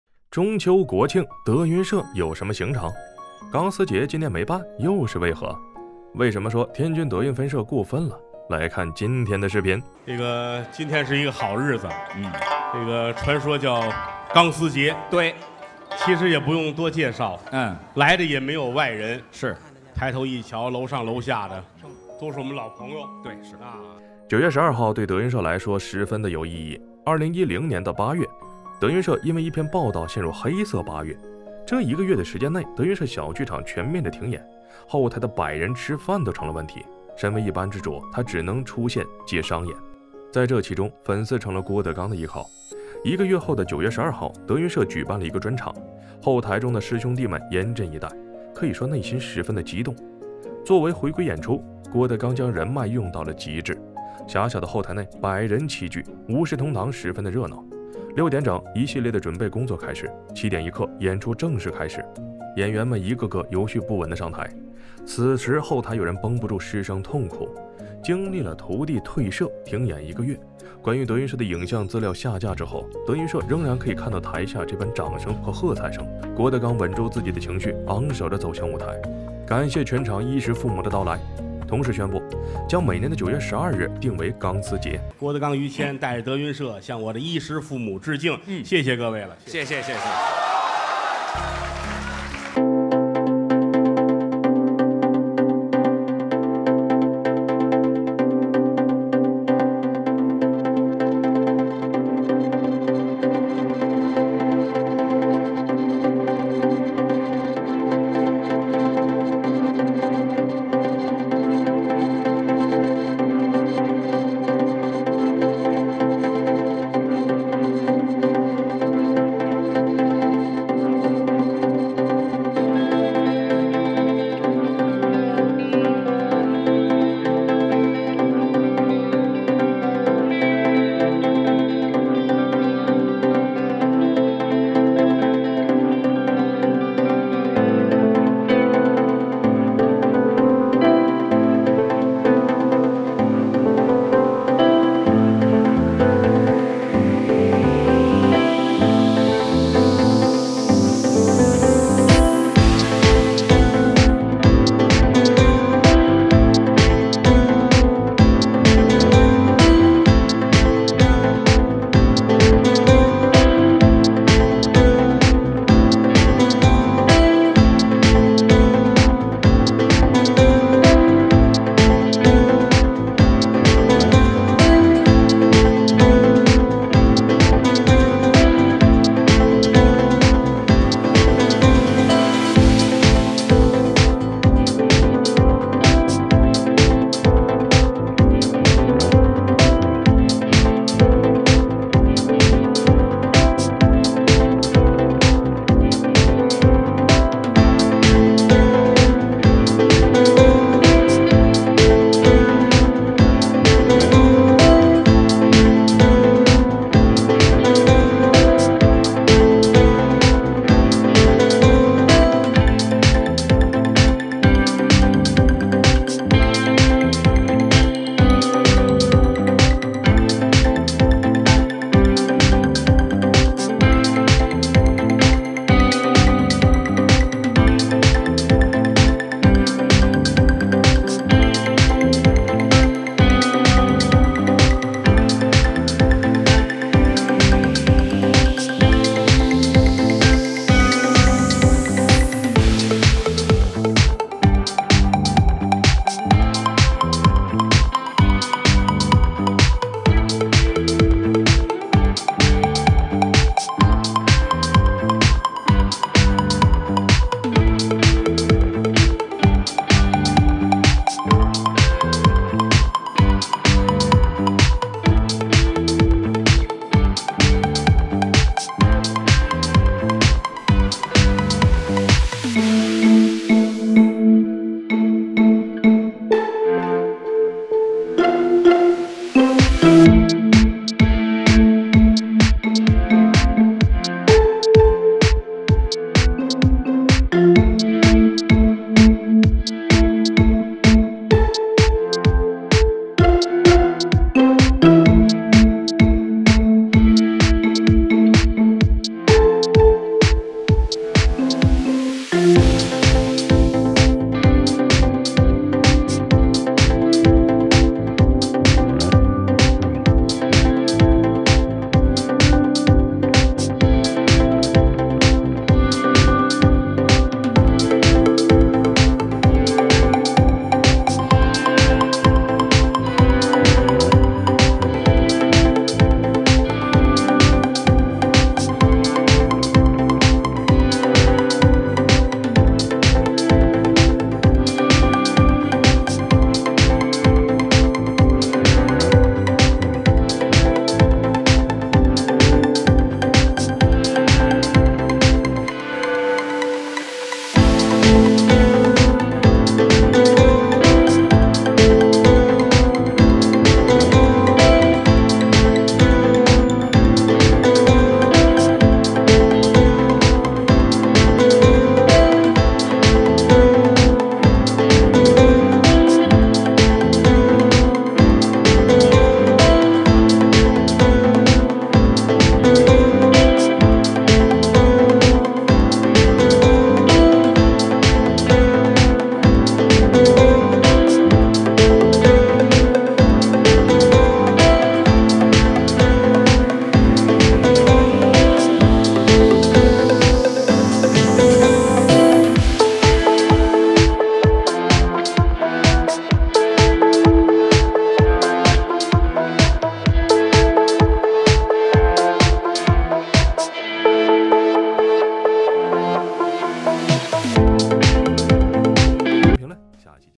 国庆中秋“双王炸”，郭德纲喜迎大将回归，与张云雷坐镇天津分社【曲艺大杂谈】
我们制作内容时会特别注意避免任何可能引起仇恨或歧视的言论或情节。 中秋、国庆德云社有什么行程？钢丝节今年没办又是为何，为什么说天津德云分社过分了？

说学逗唱，看我曲艺大杂谈！